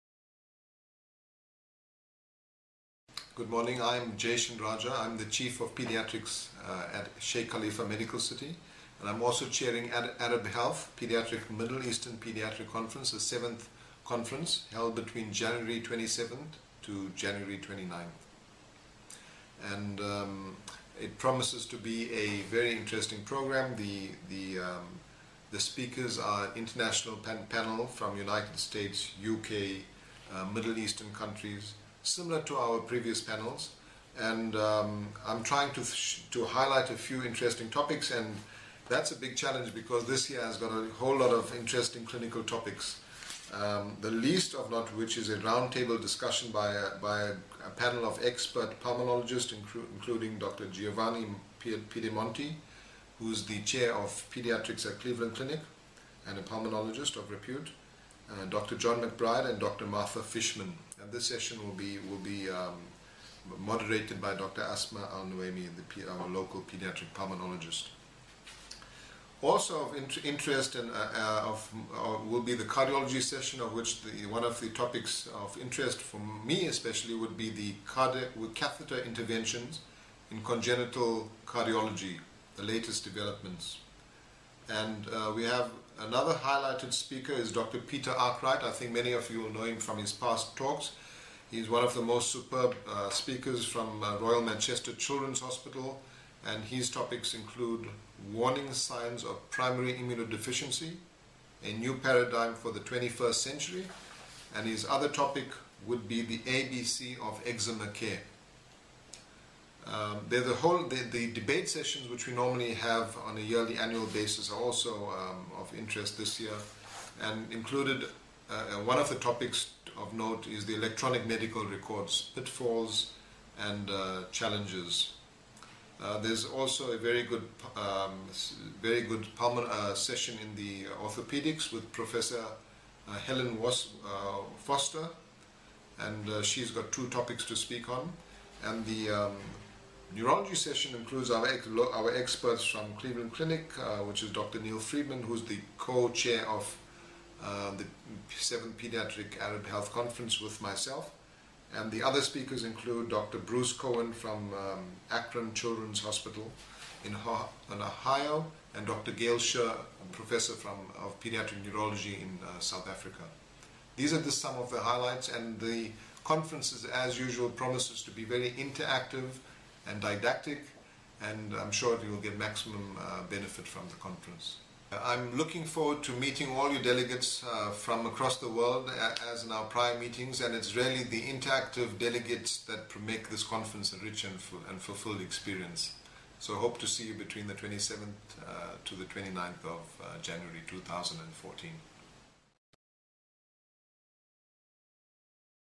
7th Middle East Paediatrics Conference at Arab Health 2014
An interview with Dr Jaishen Rajah, the chair of the Paediatrics Conference happening 27-29 January 2014 during the Arab Health Congress, Dubai.

Paediatrics, as a medical specialty, is encompassing of a number of other specialities of medicine. Along with this, there is the constant growth of paediatric diseases that are being contracted on a daily basis. With a large number of expatriate and national children in the Middle East, paediatricians are faced with a variety of different issues, complications and diseases with their patients.

The 7th Middle East Paediatrics Conference is the must attend paediatrics conference of the year, featuring 12 different sessions over the course of three days. Expert doctors from the USA, UK and the Middle East come together to discuss the latest topics, as well as to share the most up-to-date information on the management of different paediatric diseases and conditions.